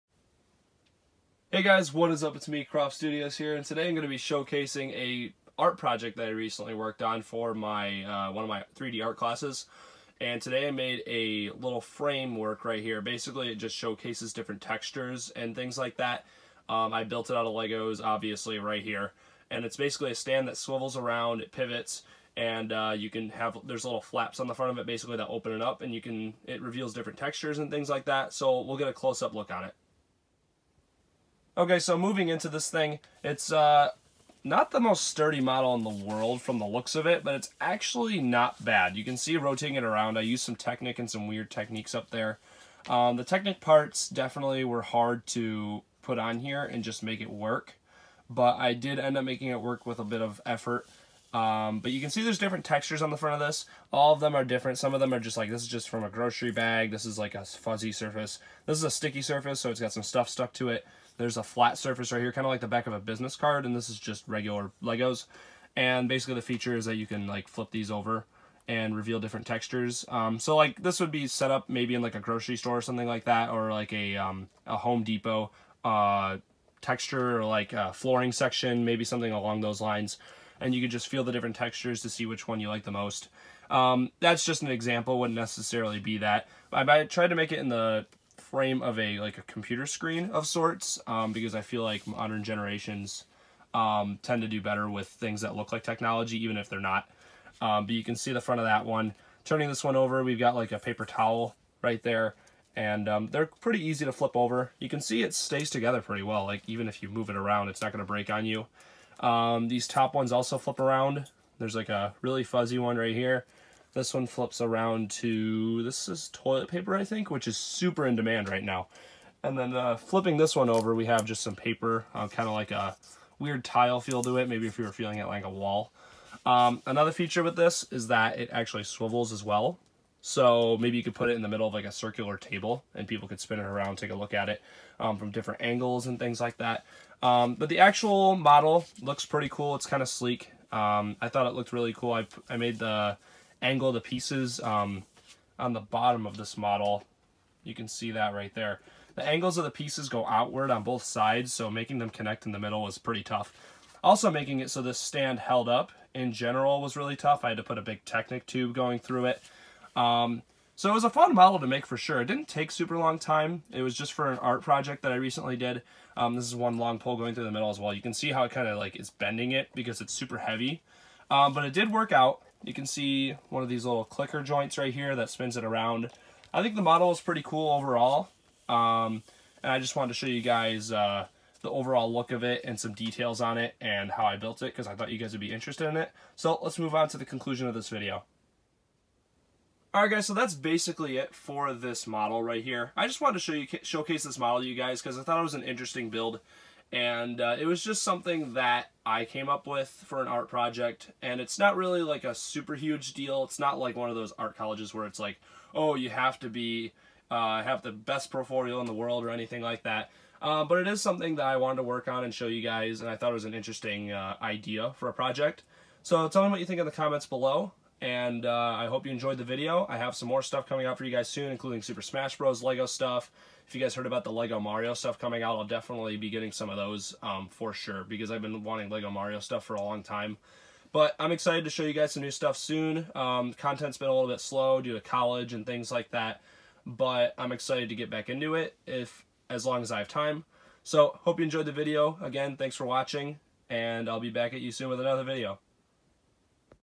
LEGO In Store Display MOC
This is a LEGO creation I made as an art project for my class!